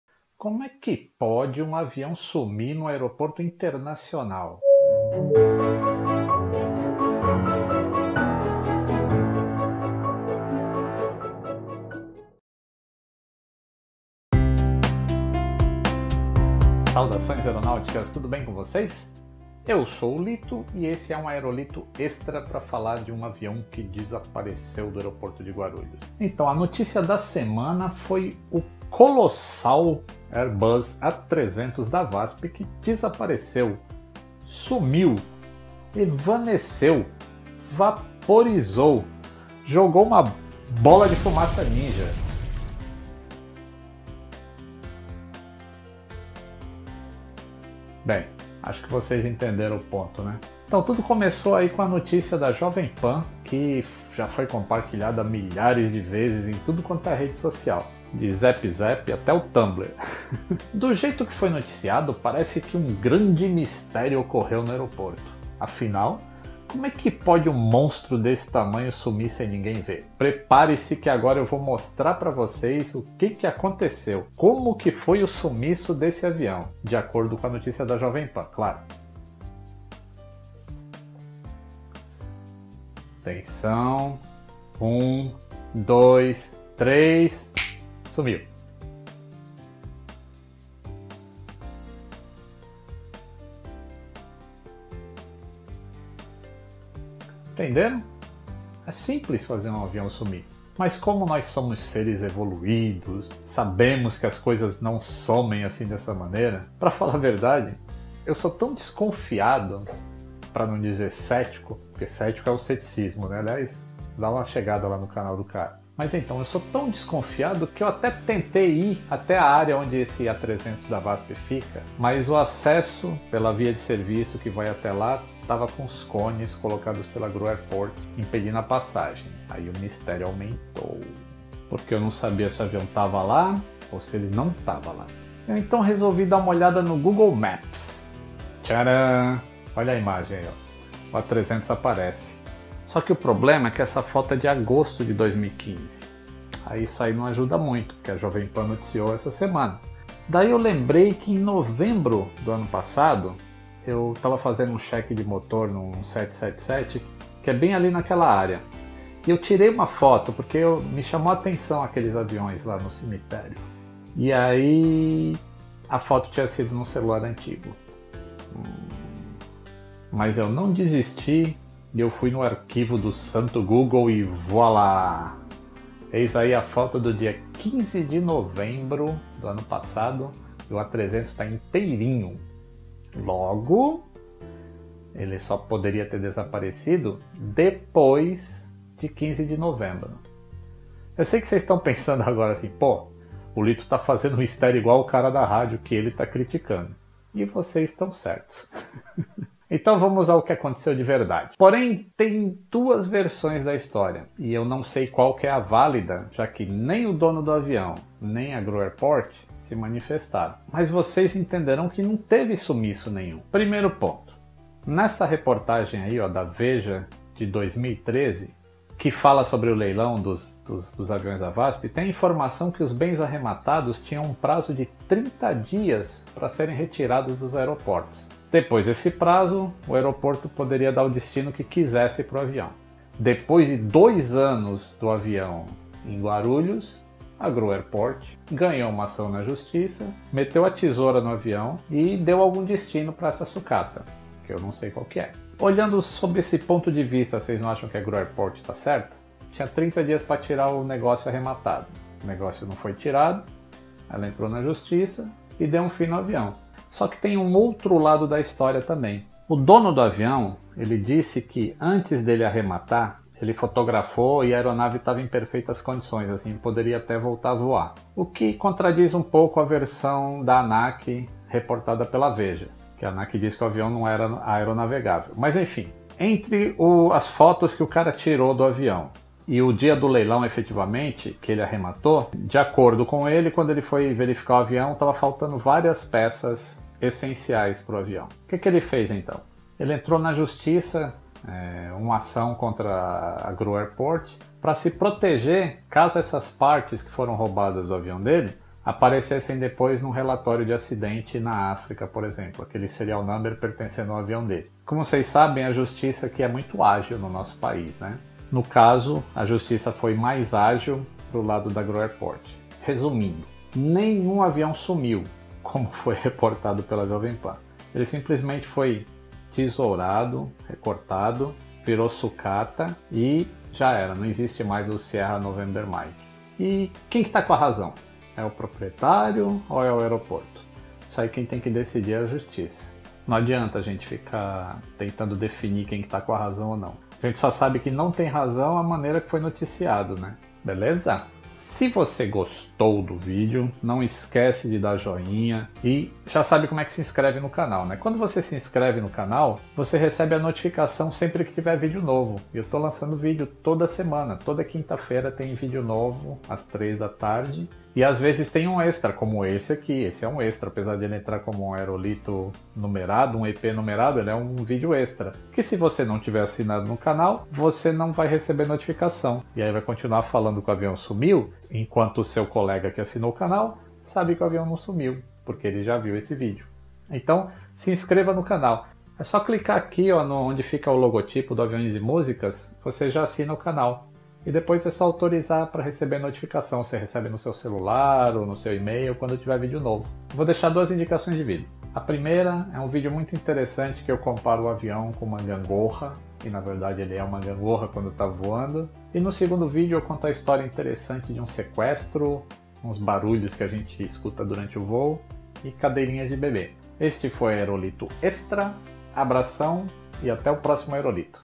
É VERDADE QUE SUMIU UM AVIÃO EM GUARULHOS? EP. 50
Como pode um avião gigantesco da VASP sumir de um aeroporto sem ninguém ver? Será que foi isso mesmo que aconteceu? A Jovem Pan noticiou o sumiço de um avião em Guarulhos, mas não foi bem isso que aconteceu.

Vídeos novos toda Quinta às 15h00. Vídeos novos quase toda segunda ou um Extra a qualquer dia.

Joselito Souza / Joselito Sousa